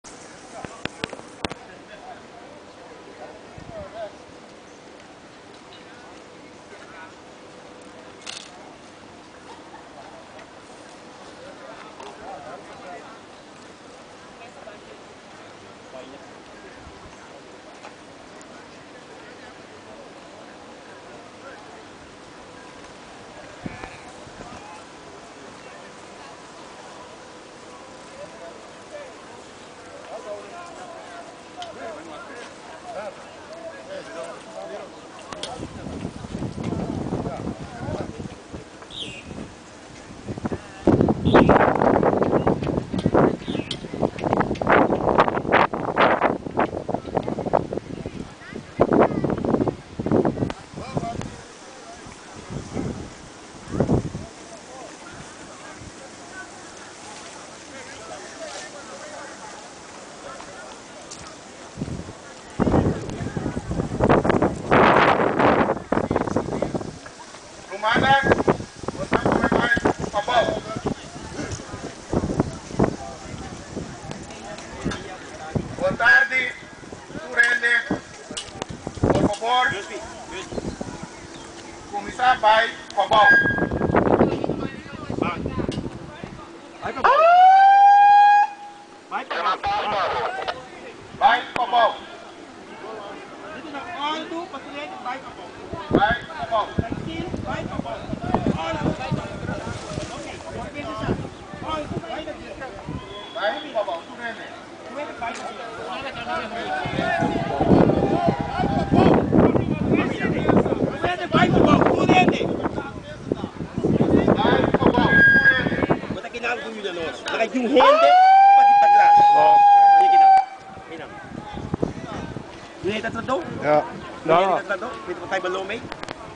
VKC RIOT TEAM - November 2007 @ Marriott Ronde Tafel Conferentie (1)
VKC ta drenta akson kontra di demonstrante nan ku tabata blokia kaya di piscadera na altura di marriott hotel